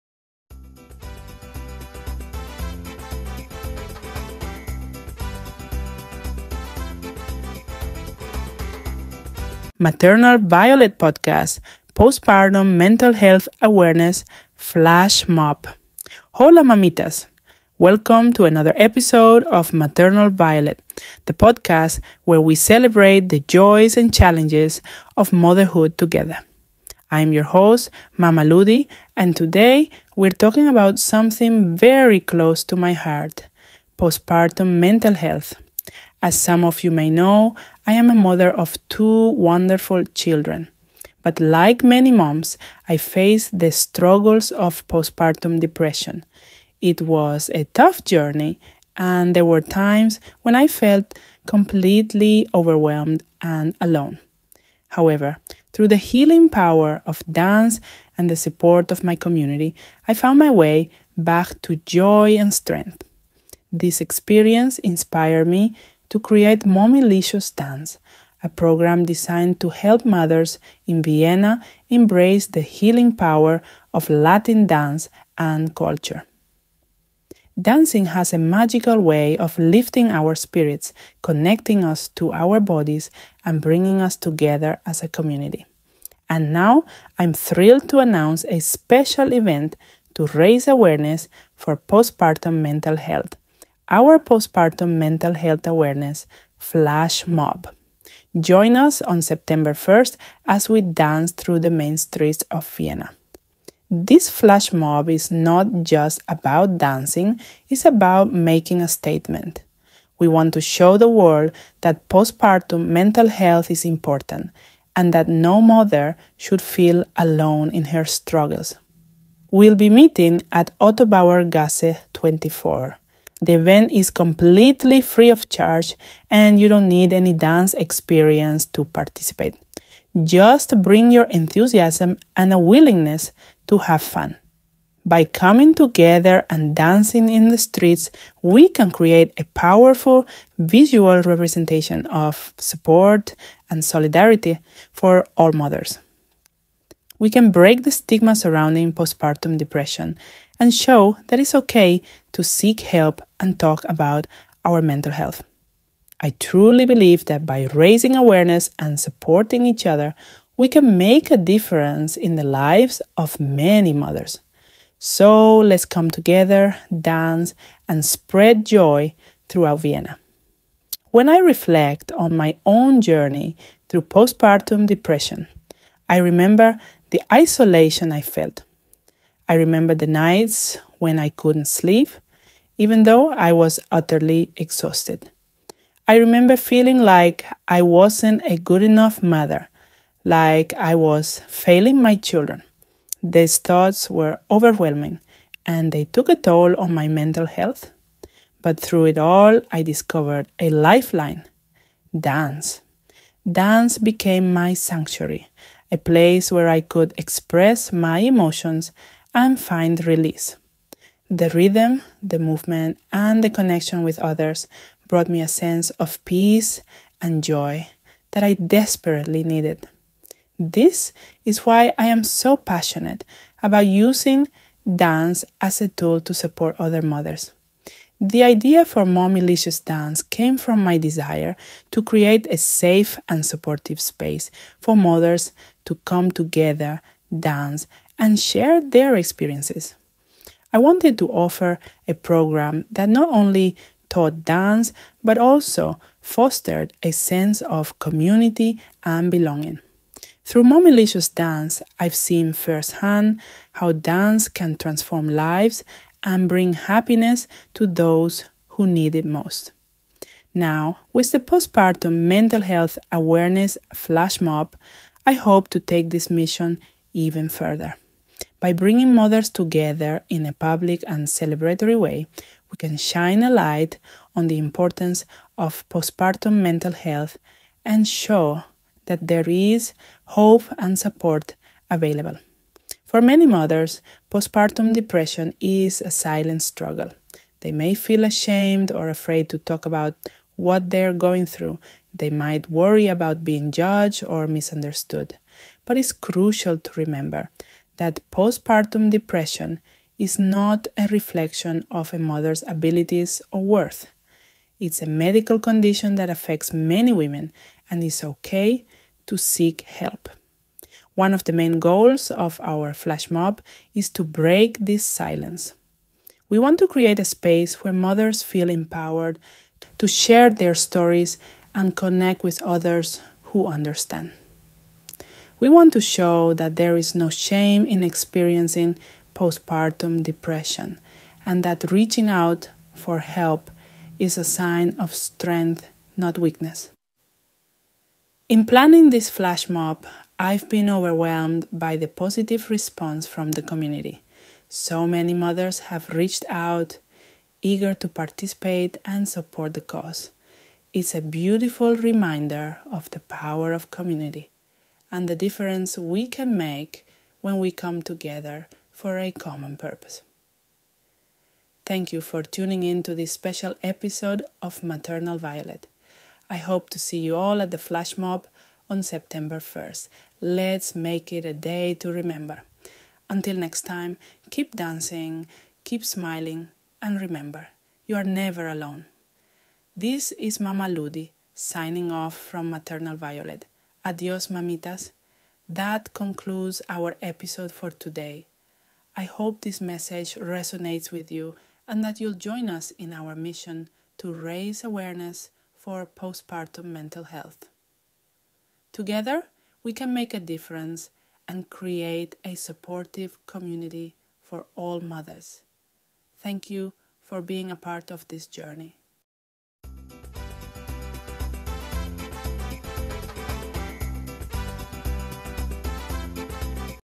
Episode 3: Postpartum Mental Health Awareness Flash Mob by Mommylicious Dance Vienna
Hola Mamitas! This is Mamá Ludi, and welcome to Maternal Violet. Today, I want to talk about our upcoming event: the Postpartum Mental Health Awareness Flash Mob. As a mom who experienced postpartum depression, I know firsthand how challenging those early months can be. That's why I founded Mommylicious Dance, to bring happiness and joy through the healing power of Latin dance and culture.

On September 1st, we’re meeting at Otto-Bauer-Gasse 24 to dance together and raise awareness for postpartum mental health. This flash mob is not just about dancing; it’s about building a supportive community and showing every mom in Vienna that they’re not alone. The event is free of charge, so bring your baby in a carrier and join us for a morning of fun, connection, and empowerment.

Let’s dance, laugh, and spread joy together! Follow us for more details and let’s make a difference. Remember, a happy mom means a happy baby. See you there, mamitas! 💜🕺💃